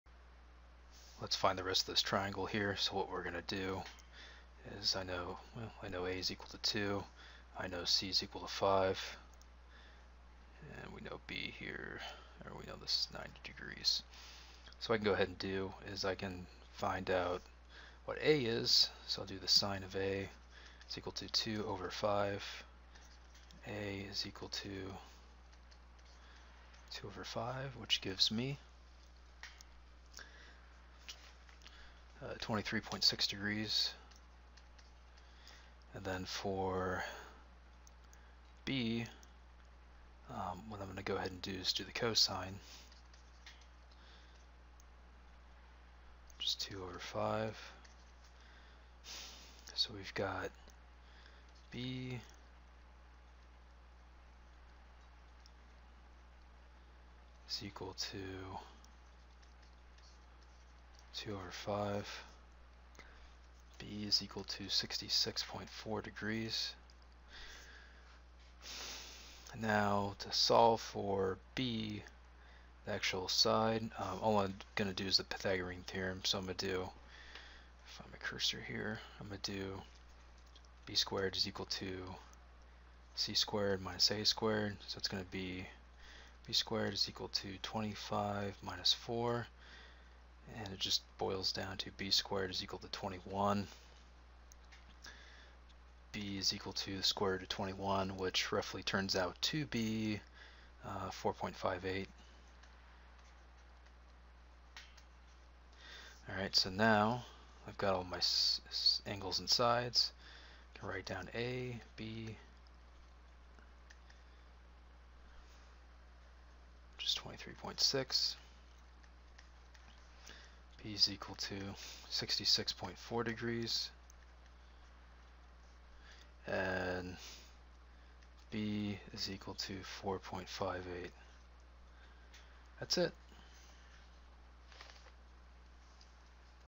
a = 2, c = 5, find b, A, and B for the right triangle
Using trig properties find the right triangle properties.